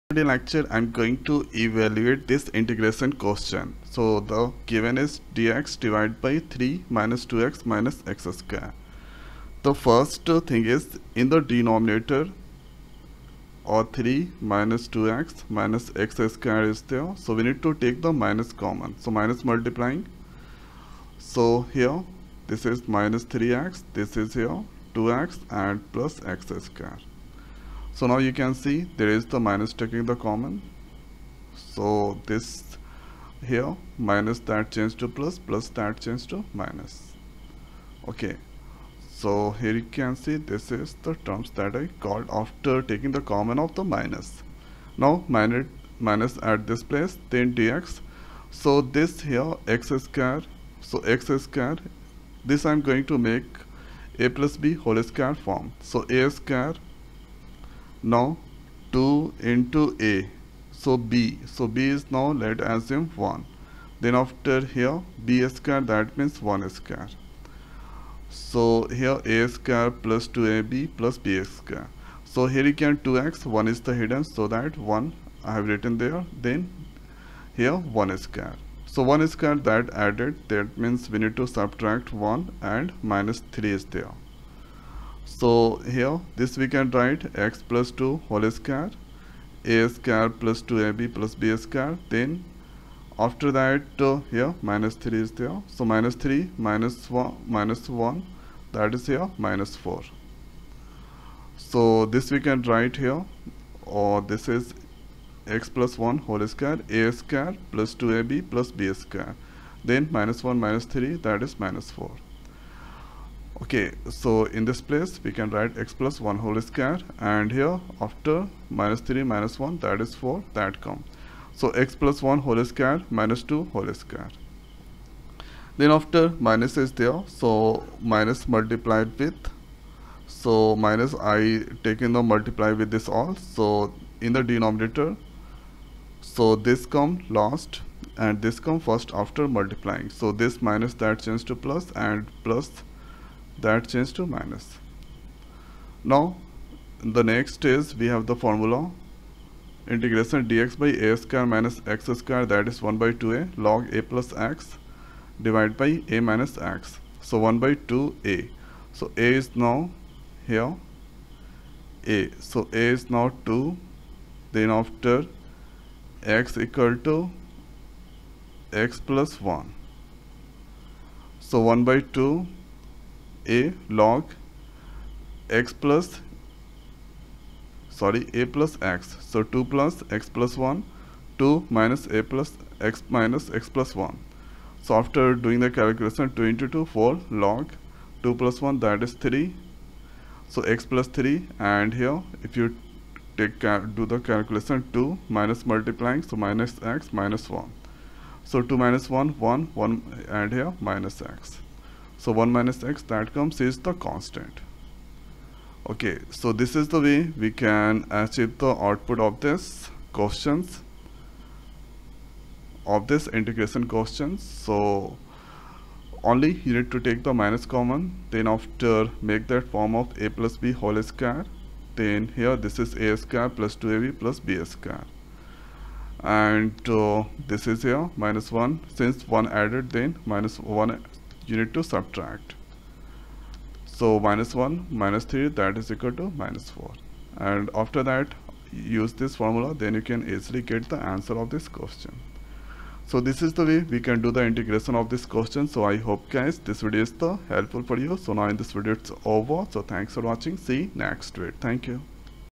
Evaluate: ∫ dx/(3 - 2x - x²)
integrate dx/3-2x-x^2
Evaluate: ∫dx/(3-2x-x^2)
Solved integral of [1/(3-2x-x^2)dx
Find integral dx/ 3 - 2x - x^2
find integration of ∫dx/(3-2x-x^2)